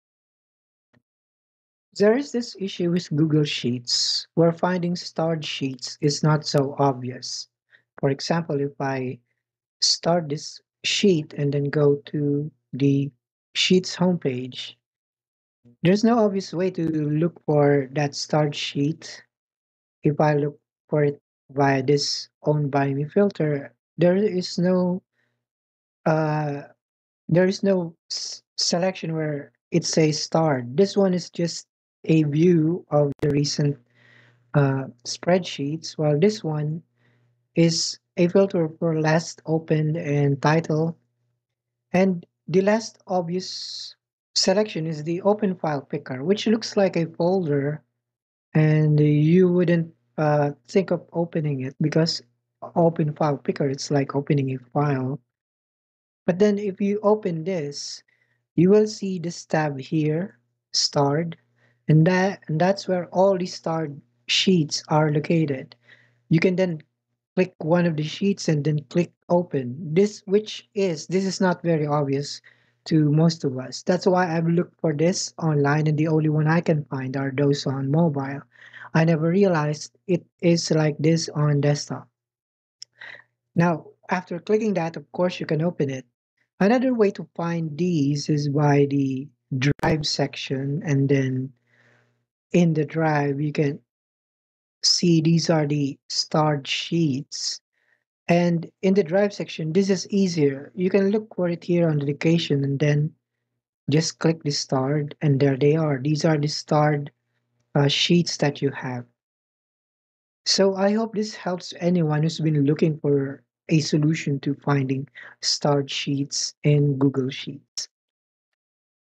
How to find the starred sheets in Google Sheets (Desktop)
How in the world do I find the starred sheets in Google Sheets on my desktop browser?

I was struggling to find this out a while ago. I've scoured the internet trying to figure it out and realized the filter was hiding from plain sight.

Hint: You will never look at a folder icon the same way again.